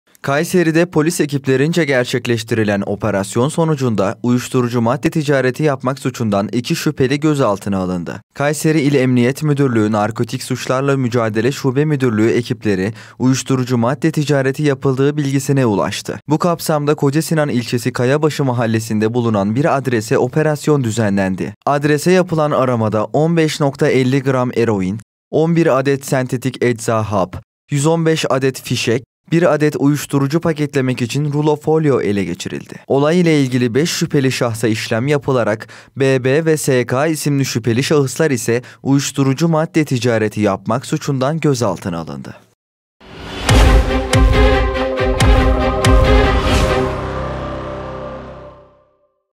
Uyuşturucu Madde Operasyonunda 2 Gözaltı
KAYSERİ (1HA) - Kayseri’de polis ekiplerince gerçekleştirilen operasyon sonucunda
‘uyuşturucu madde ticareti yapmak’ suçundan 2 şüpheli gözaltına alındı.

Kayseri İl Emniyet Müdürlüğü Narkotik Suçlarla Mücadele Şube Müdürlüğü ekipleri 'uyuşturucu madde ticareti' yapıldığı bilgisine ulaştı. Bu kapsamda Kocasinan ilçesi Kayabaşı Mahallesinde bulunan bir adrese operasyon düzenlendi. Adrese yapılan aramada 15.50 gram eroin, 11 adet sentetik ecza hap, 115 adet fişek, 1 adet uyuşturucu paketlemek için rulo folyo ele geçirildi.

Olay ile ilgili 5 şüpheli şahsa işlem yapılarak, B.B. ve S.K. isimli şüpheli şahıslar ise ‘uyuşturucu madde ticareti yapmak’ suçundan gözaltına alındı.